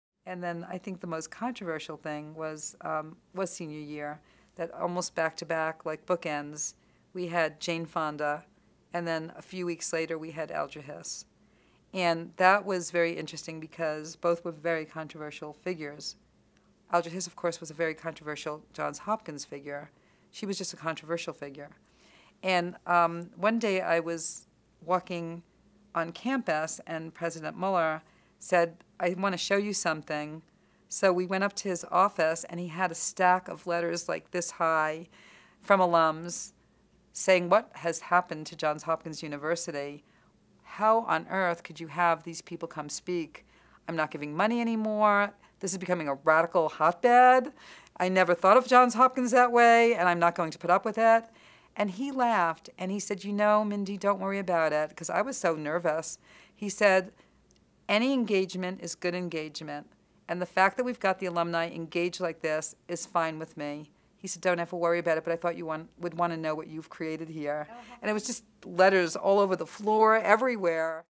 Mindy Farber on bringing controversial figures to speak on campus as a student
Mindy Farber (A&S '74) describes her work bring controversial figures to speak on campus in the 1970s, and University President Steven Muller's surprising response to negative feedback from alumni.